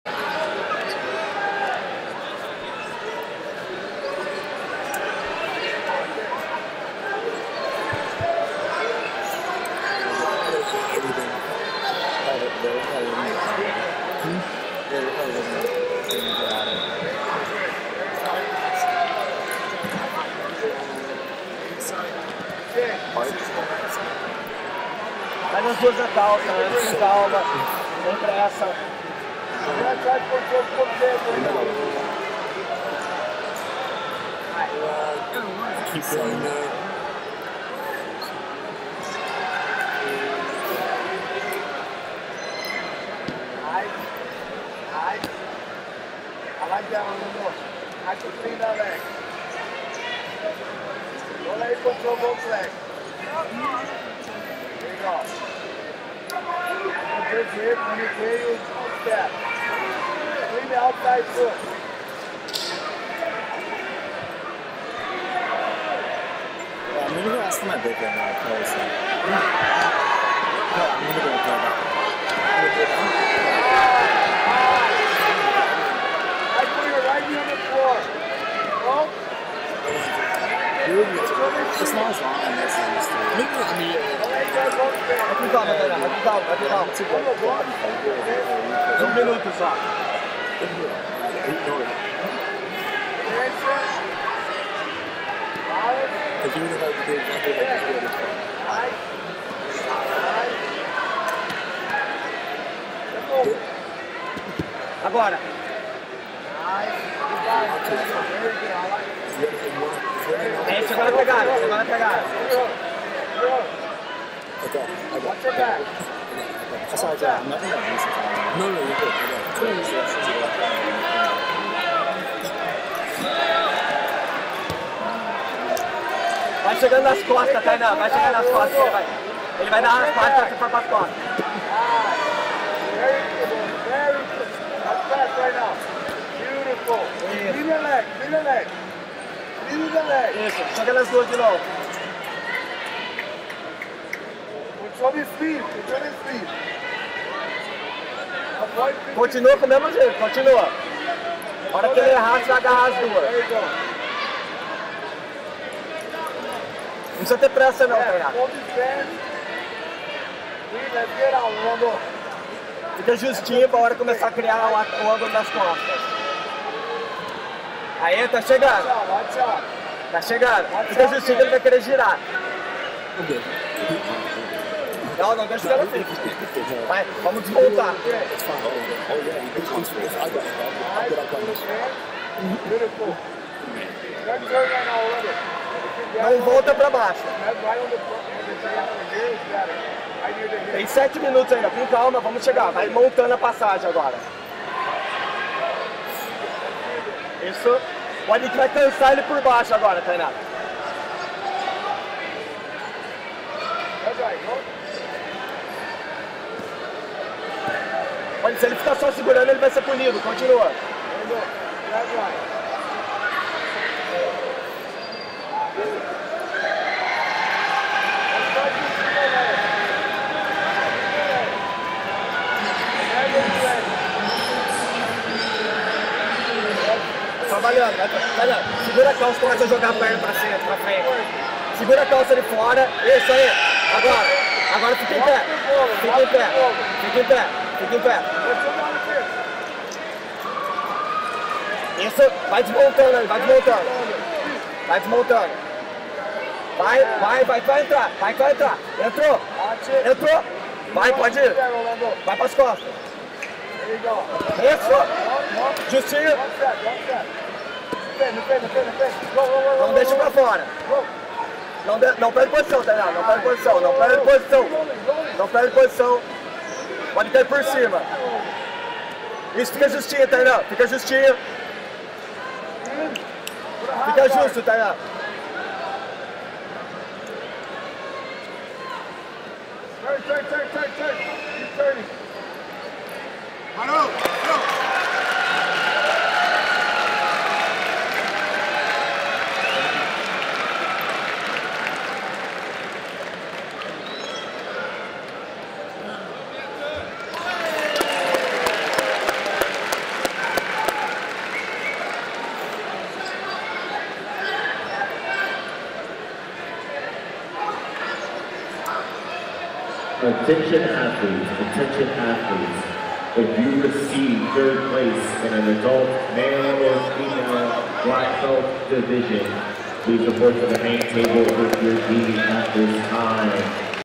Tainan Dalpra vs Rolando Samson | 2023 IBJJF Pan Championship
Tainan Dalpra and Rolando Samson meet in the semifinals of the middleweight division at the 2023 IBJJF Pan Championship!